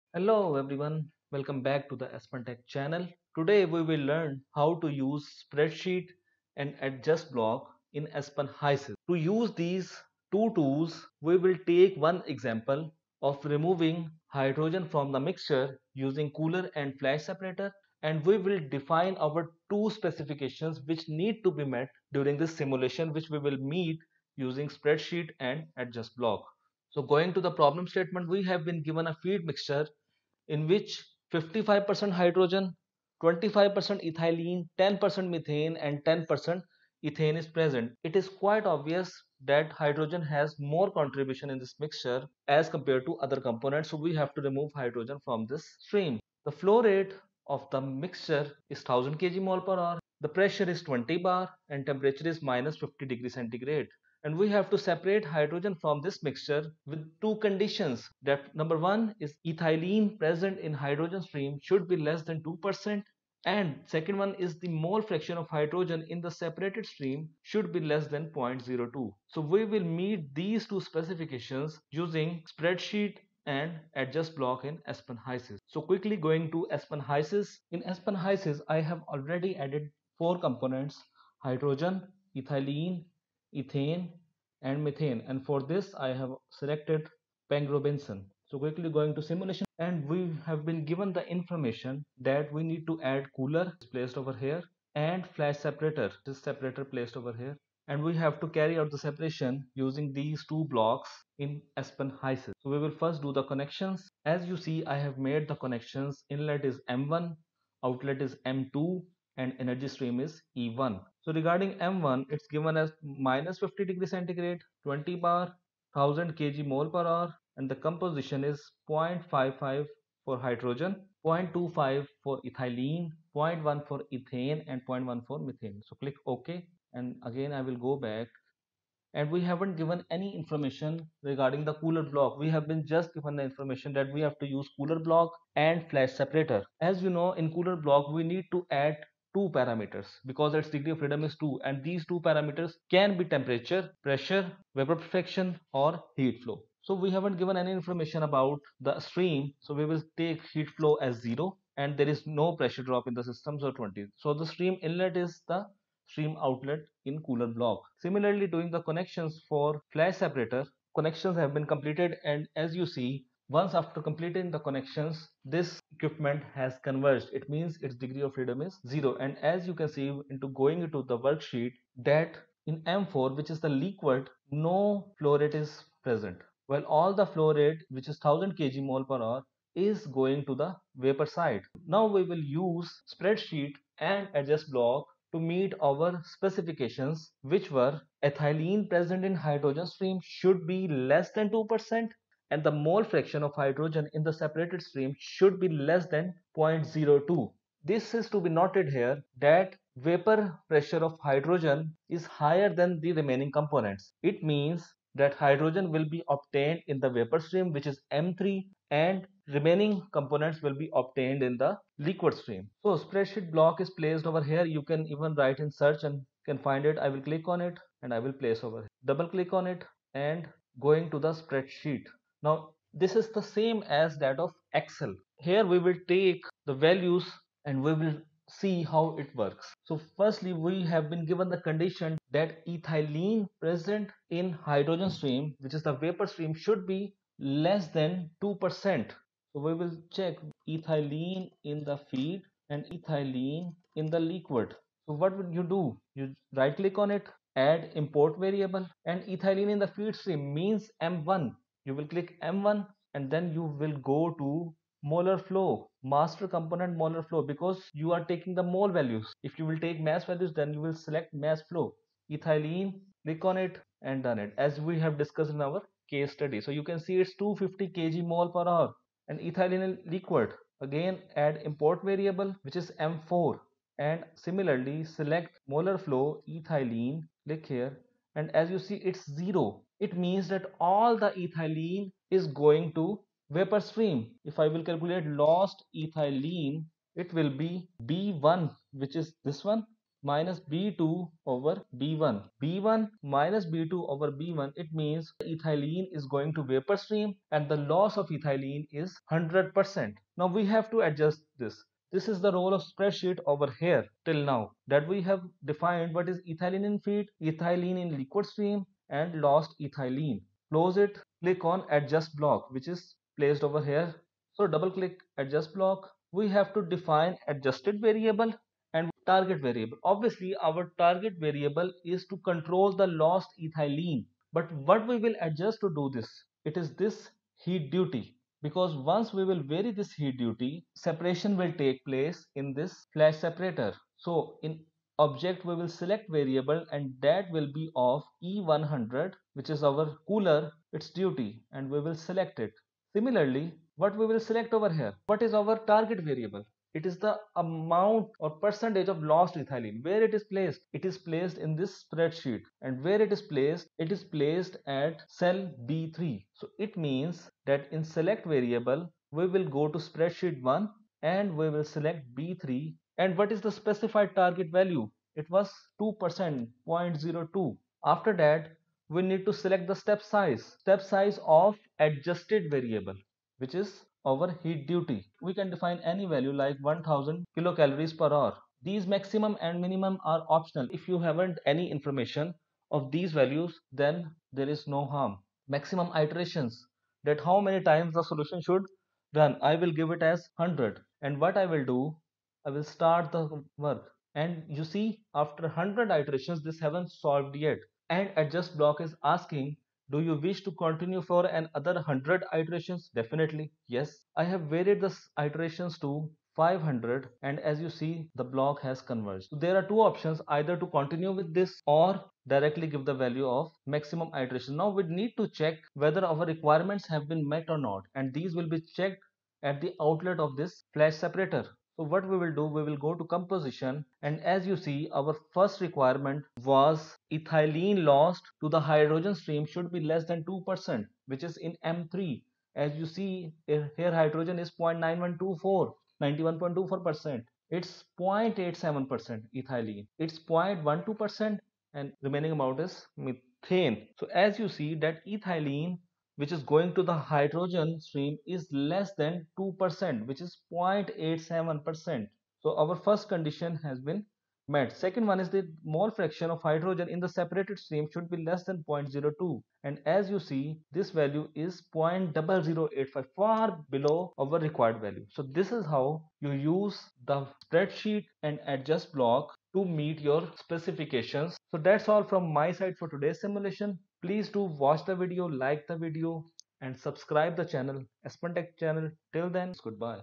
Use Spreadsheet and Adjust tools in Aspen HYSYS - Lecture # 14
Learn to use spreadsheet and adjust tools to meet specifications of your under-study separation process in Aspen HYSYS. This tutorial is focused on removal of hydrogen stream from the mixture, and the required specifications are met using these two tools.

Problem Statement:

The feed stream contains 55 mol % hydrogen, 25 mol % ethylene, 10 mol % methane and 10 mol % ethane. The flowrate of feedstream is 1000 kgmole/h. The feed is entering the cooler at -50C and 20 bar. The objective of the simulation is to remove hydrogen from the mixture (using cooler and separator) under two conditions:

1. Ethylene present in hydrogen stream should be less than 2%.
2. The mole fraction of hydrogen in the separated stream should be less than 0.02.